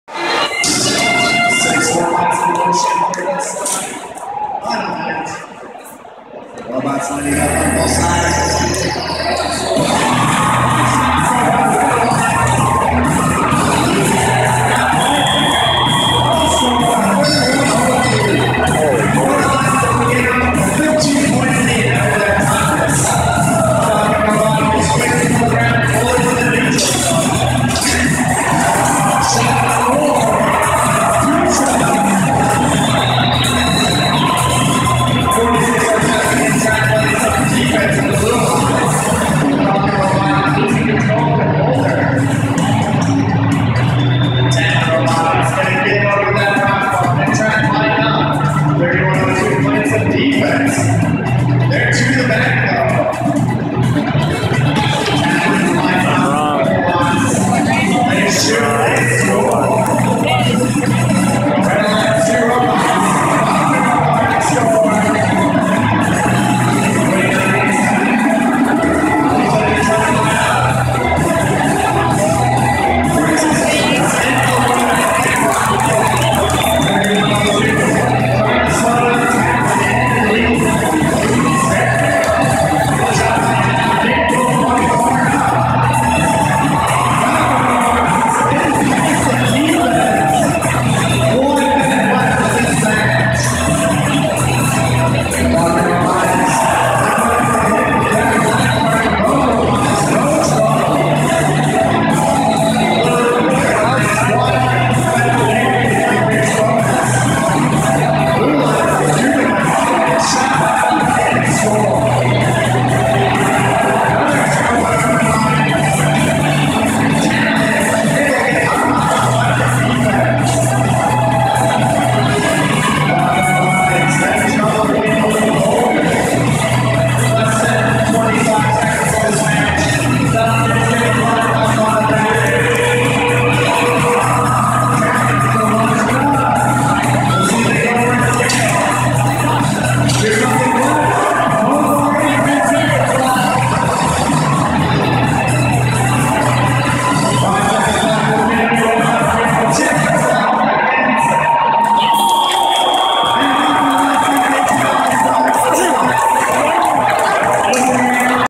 2016 MN State Semis 3rd Place Match
Minnesota State High School League State Championships 2016 Semifinals Match for 3rd Place
(FIRST Robotics)